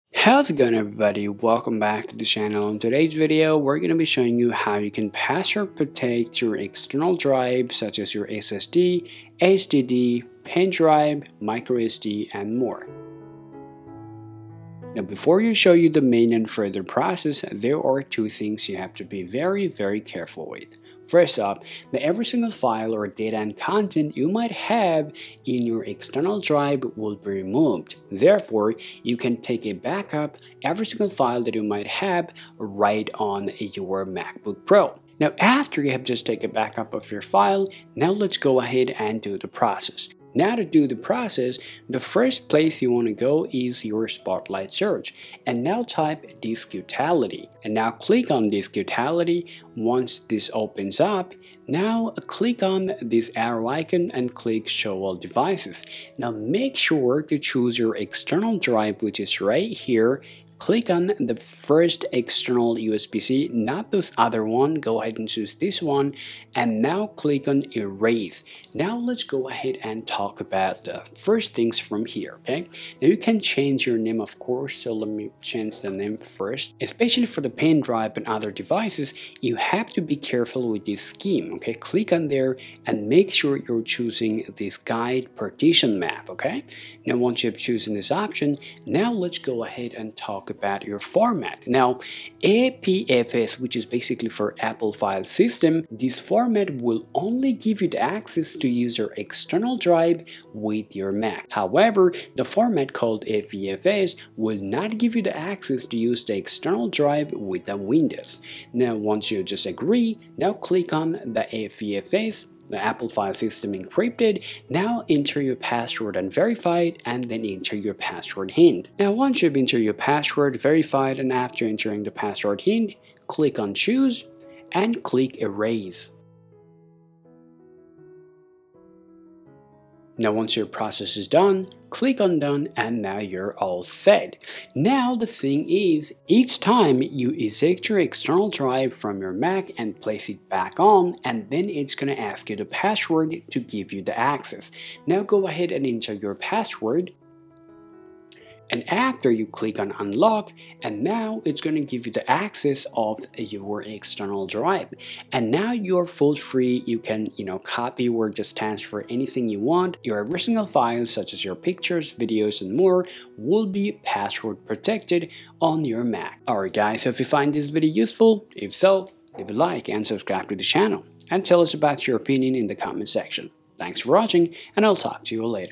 How To Password Protect External Hard Disk [Mac M1]
You can Protect your important photo,video,audio,documents and files with a password on an external drive (USB Flash Drive/ Hard Drive/ SSD/ Micro SD) from your M1 Mac, to keep them safe? The step-by-step process to password protect an external hard drive from M1 MacBook Pro/Air, Mac Mini, or iMac.

0:00 What this video is about
0:15 keep a Backup of all files in the external drive
0:40 Lock a USB Drive with Password